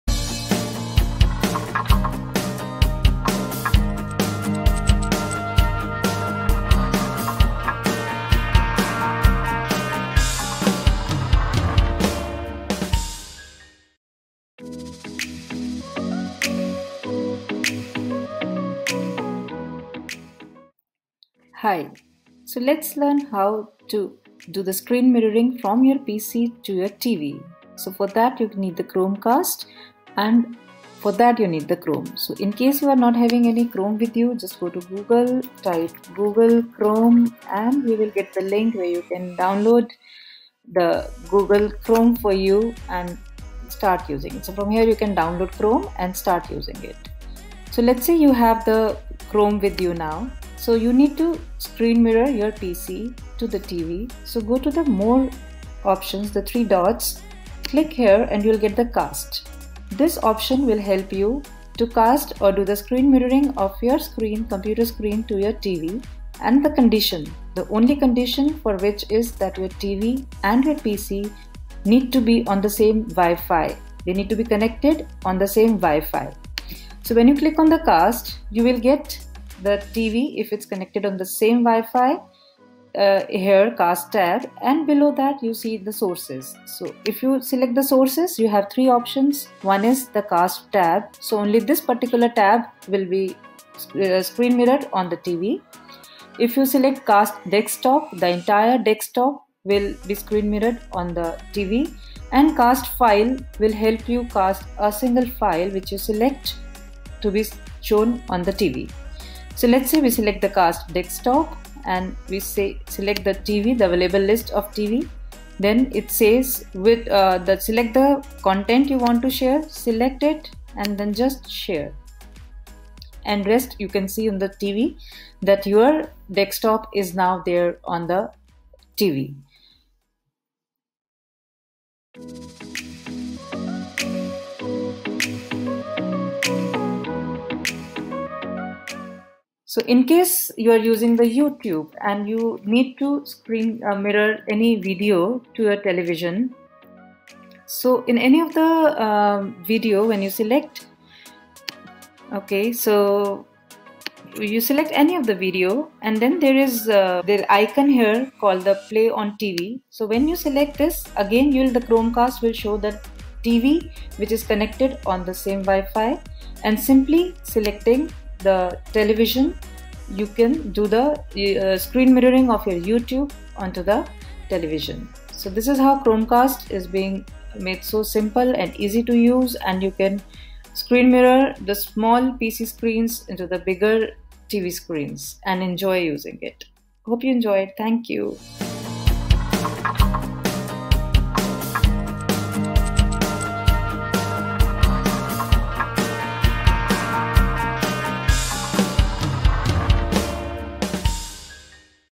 How To Tutorials: Screen Mirror PC or Laptop to TV using ChromeCast
How To Tutorial: Screen Mirror PC or Laptop to TV using ChromeCast

Pre- requisite:

1/ PC or Laptop needs to be connected on same WiFi as your TV
2/ Must have Chrome browser in your PC or Laptop